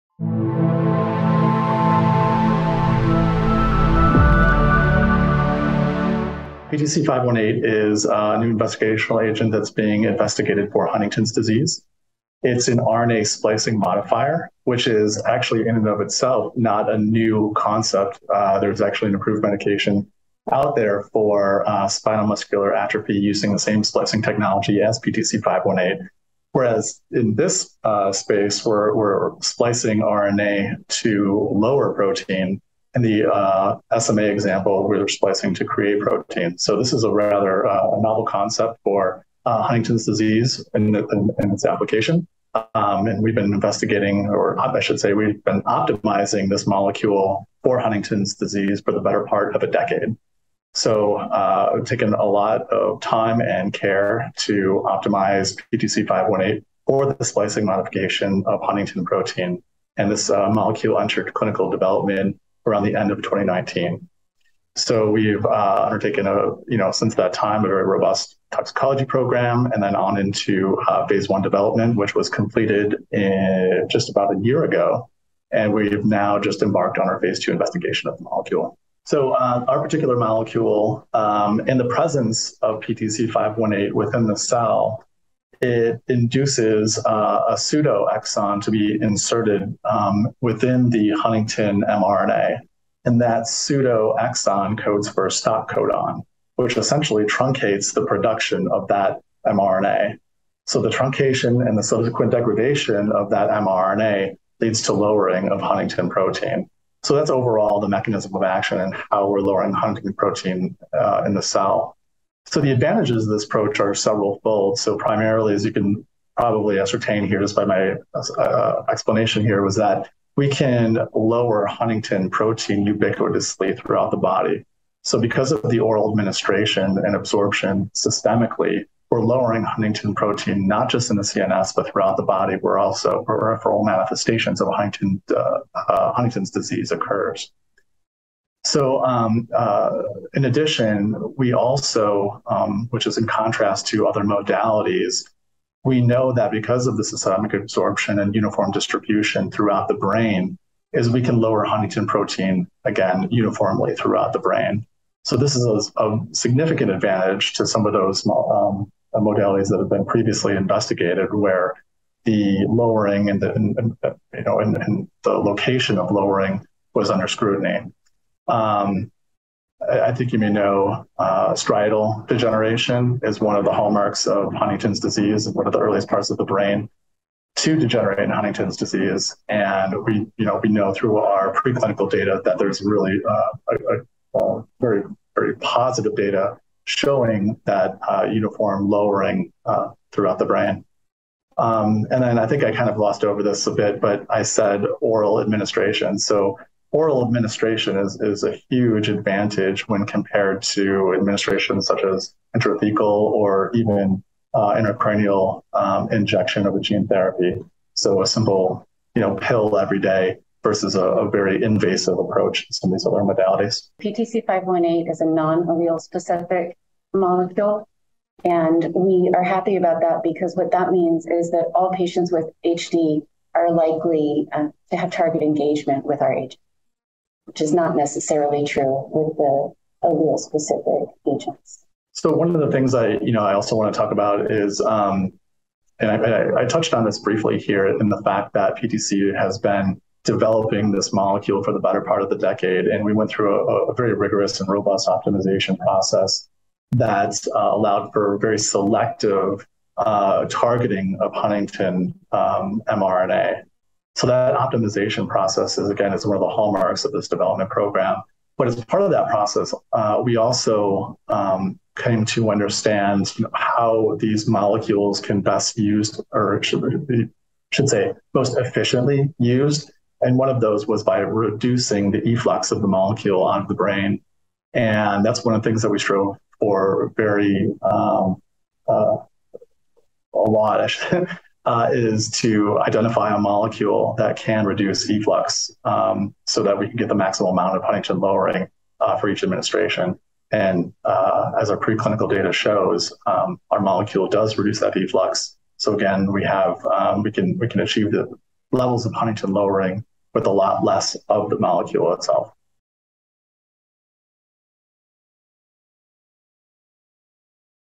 PTC518: oral splicing modifier under investigation for the treatment of Huntington’s disease
Amy-Lee Bredlau, MD; Lee Golden, MD; and Brian Beers, BSc; PTC Therapeutics, Inc., New Jersey, introduce PTC518 as a new investigational agent in the Huntington’s disease (HD) space. PTC518 is an oral RNA splicing modifier, which acts to incorporate a pseudoexon into the huntingtin (HTT) pre-mRNA, creating a nonsense mutation that truncates the mRNA production. The consequential degradation of HTT mRNA leads to lowering of HTT protein. Brian Beers highlights that PTC518 is uniformly distributed throughout the brain and the body, giving it the potential to tackle peripheral manifestations of disease as well as acting in key brain regions. Preclinical studies have demonstrated the ability of PTC518 to promote splicing and lower HTT protein levels in human cells and mouse models, in a dose-dependent manner. Additionally, efforts to reduce efflux have resulted in balanced peripheral versus brain HTT lowering, increasing the therapeutic window. PTC518 is now in Phase II development (PIVOT-HD; NCT05358717) in patients with HD ISS Stage 2 disease, after the Phase I program provided promising safety and pharmacology data, and early proof of splicing mechanism in healthy volunteers. This interview took place during the European Huntington's Disease Network 2022 Plenary Meeting.